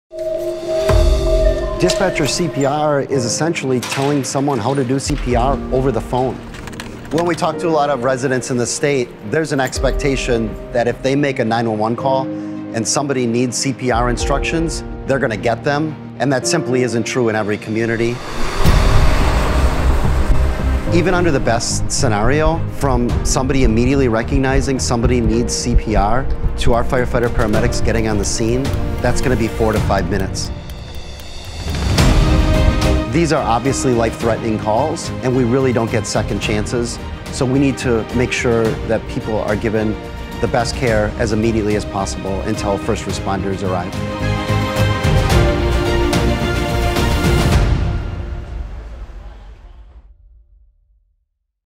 911 Dispatcher Assisted CPR PSA #1
The American Heart Association advocates for all 911 dispatchers in Wisconsin to be able to give CPR coaching over the phone. Fire Chief Jon Cohn shares why this is such an important issue that could save lives in Wisconsin.